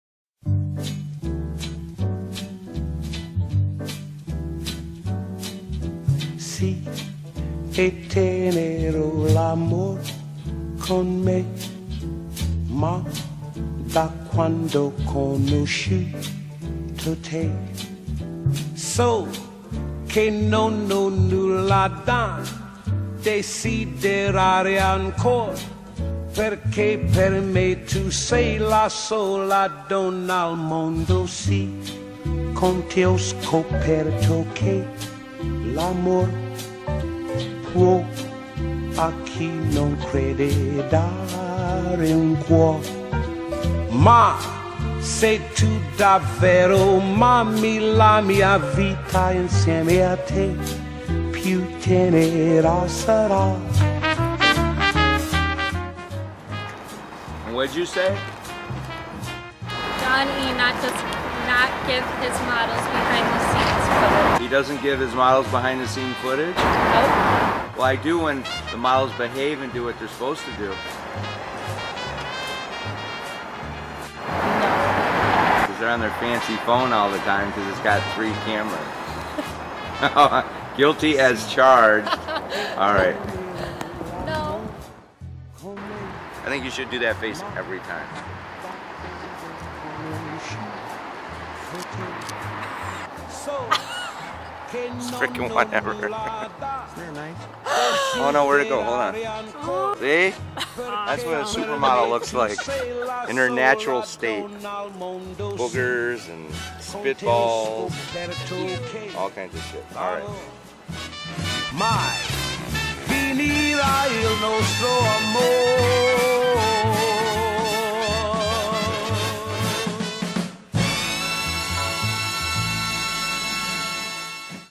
BTS - AVANTI MODA Magazine Paris Fashion Feature Photo Shoot. 2020 Spring Fashion Issue.
A quick look behind the scenes!  Model Bianca Lomedico shoots with photographer John Enot for AVANTI MODA Magazine's 2020 Spring Fashion Issue... Paris Paris!

Just a kind gesture on our part to help kick off the new year!  Ciao!